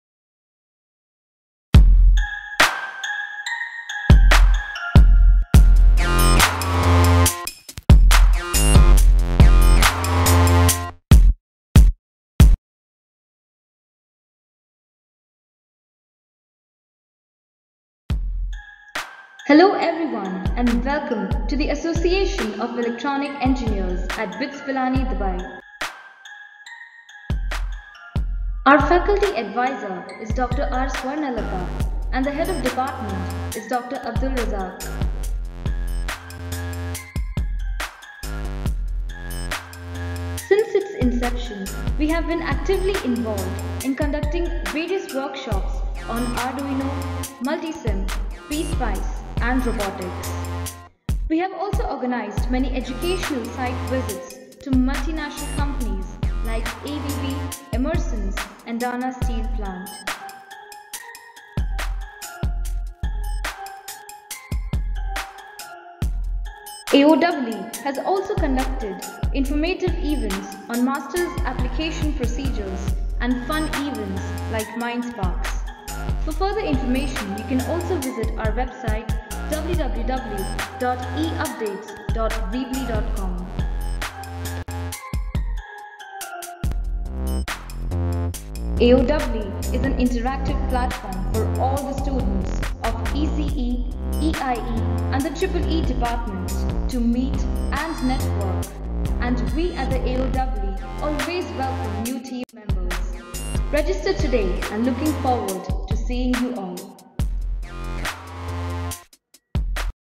BITS Pilani - Introduction to Association Of Electronic Engineers
From creative workshops to educational site visits, the Association Of Electronic Engineers is an interactive platform for EEE, ECE and EIE students to update their knowledge.